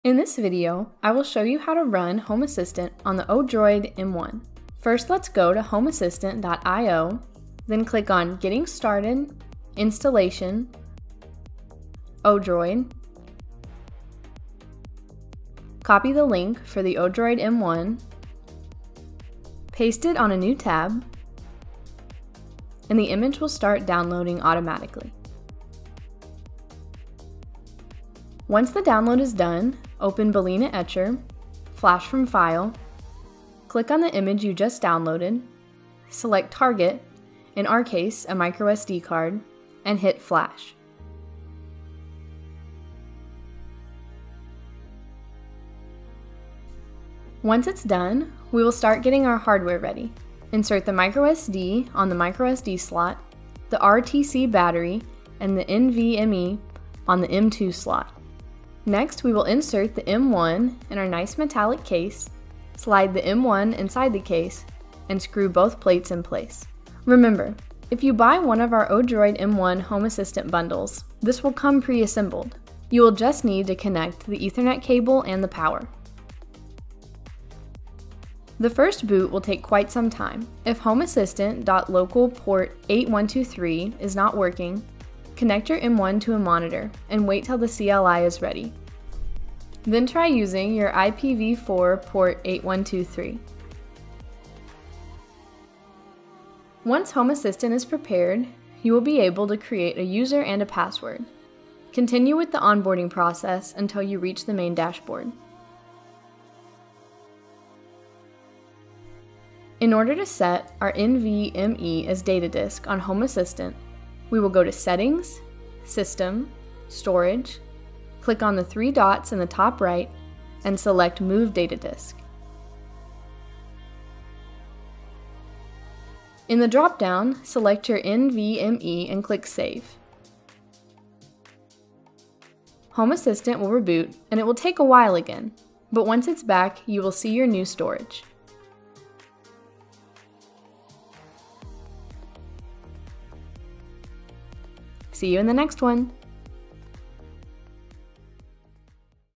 Installing Home Assistant on the ODROID-M1
In this video we show you how to flash and boot Home Assistant on an ODROID-M1 plus how to get your data move to an NVMe.

0:00 Flashing microSD with Home Assistant
0:47 Assembling Hardware
1:19 Booting Home Assistant
1:49 Moving Data to NVMe